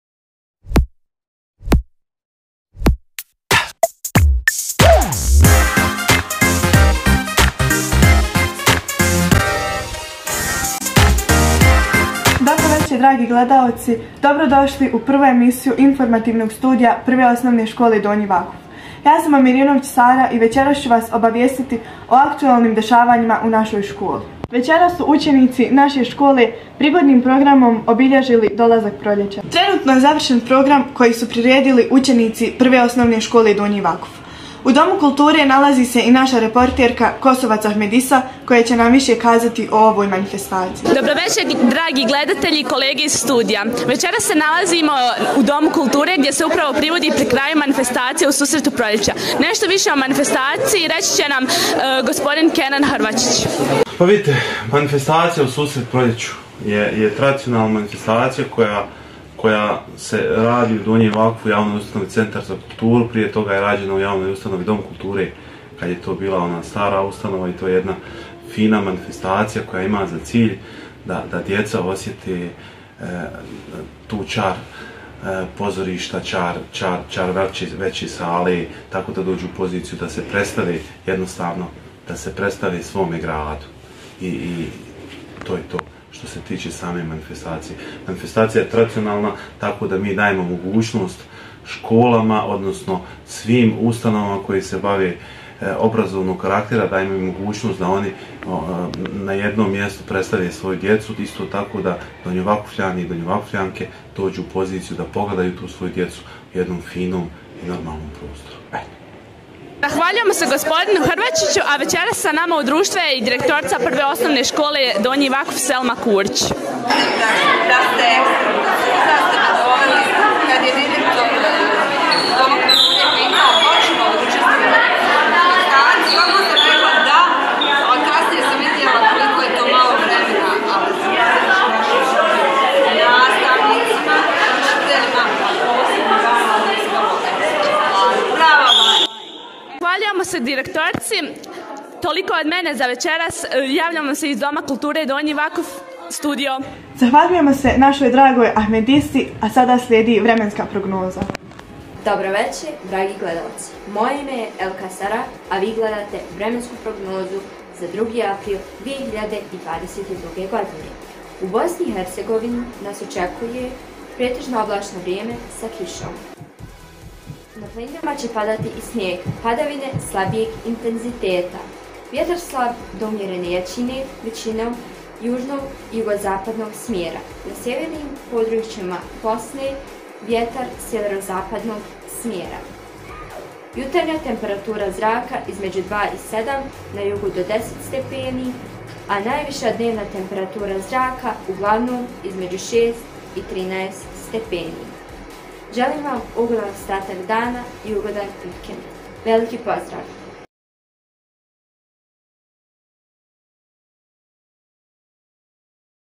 1. Informativna emisija - Informativni studio Prve OŠ Donji Vakuf
Dobro došli u prvu emisiju Informativnog studija Prve OŠ Donji Vakuf!

Organizatorica: prof. bosanskog jezika i književnosti Azra Talam Habib
Voditeljica: Sara Omerinović
Voditeljica 2: Sarah Elkaz
Reporterka: Ahmedisa Kosovac
Snimateljica: Džejla Dervišić
Montaža: Sarah Elkaz

Veliko hvala direktoru Centra za kulturu, javno informisanje i sport Donji Vakuf Kenanu Hrvačiću i direktorici Prve OŠ Donji Vakuf Selmi Kurić.